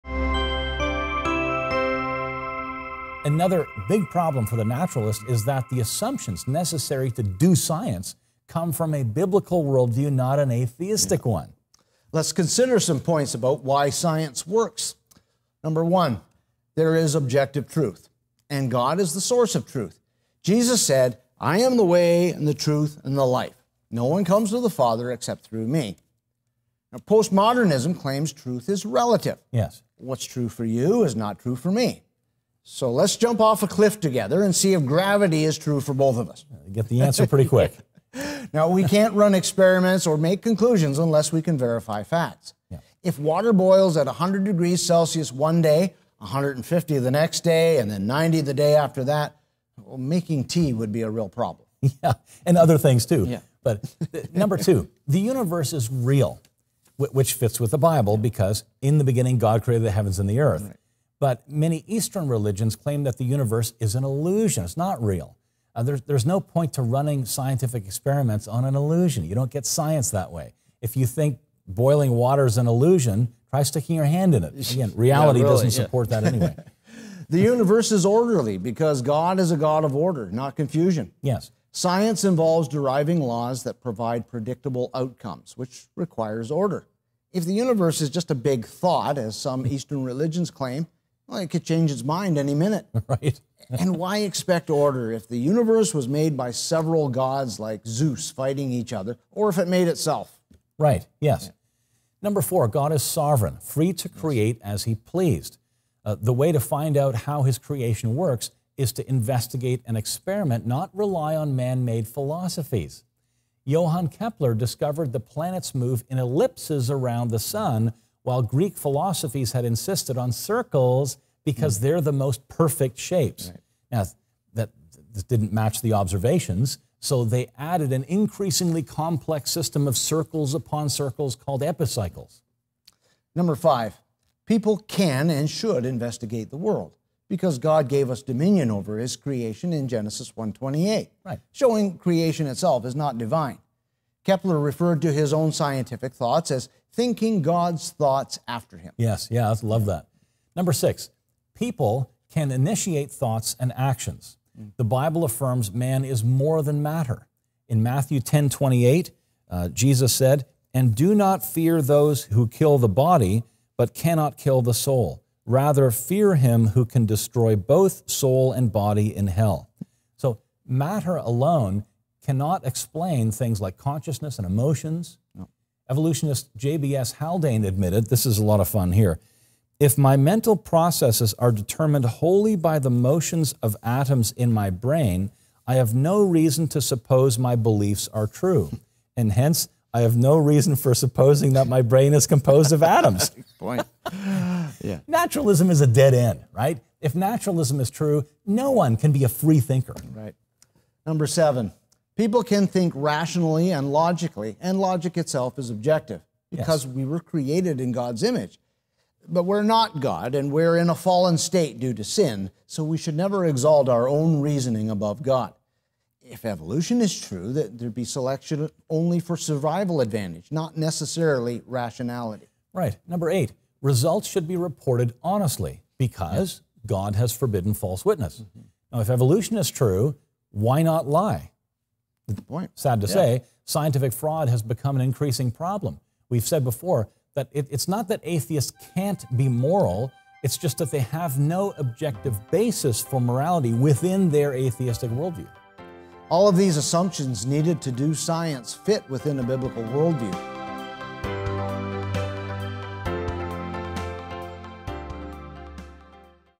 Eight assumptions foundational to science
Christians can't do science? Actually, the Christian worldview is the only one that provides a foundation for the assumptions required to do science.